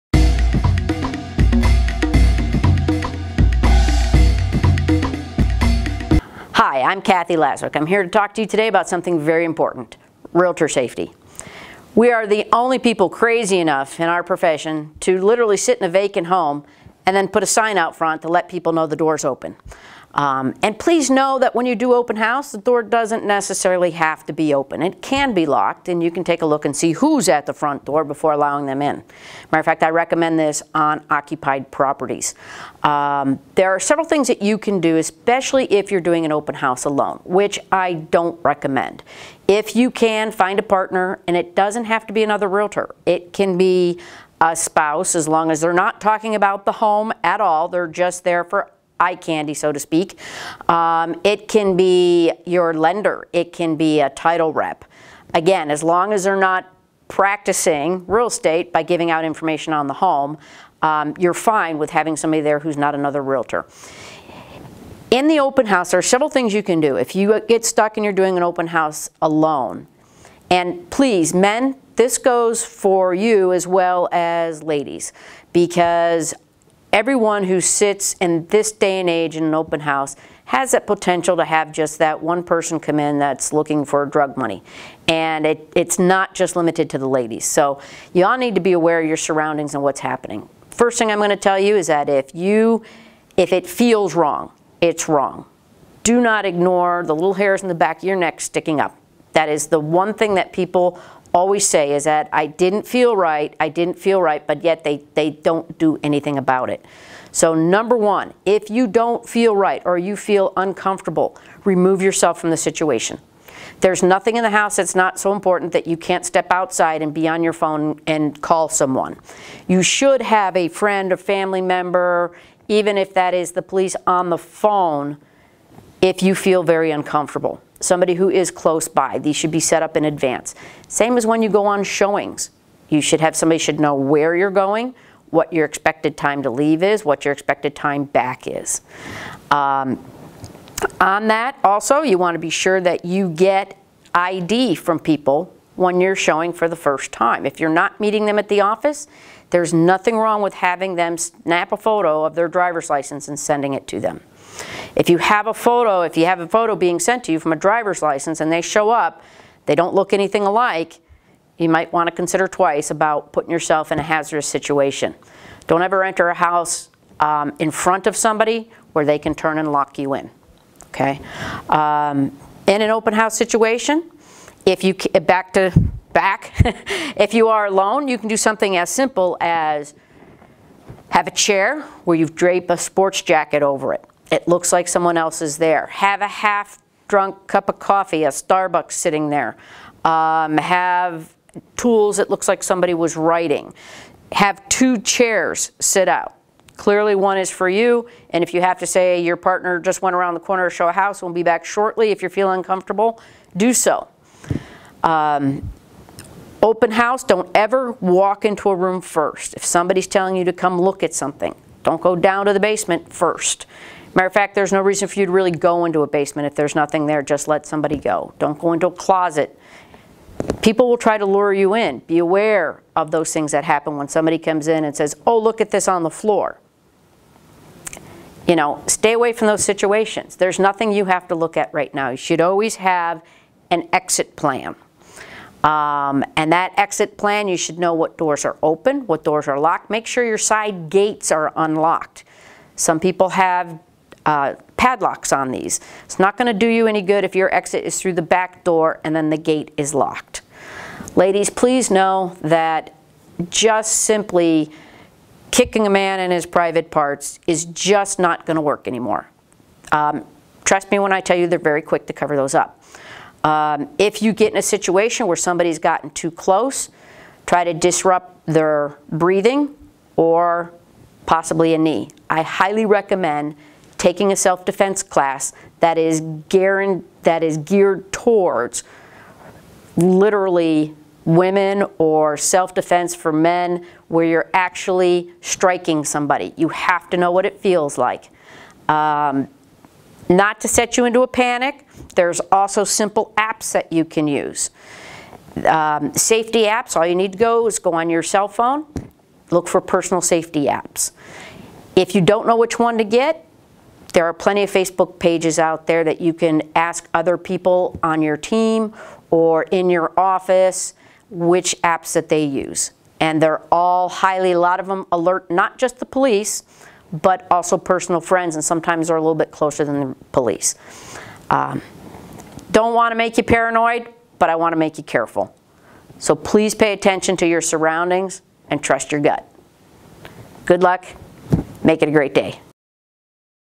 Start Smart with Kathy Laswick
Start Smart: Resources for New REALTORS®

Kathy Laswick gives tips and best practices on REALTOR® safety.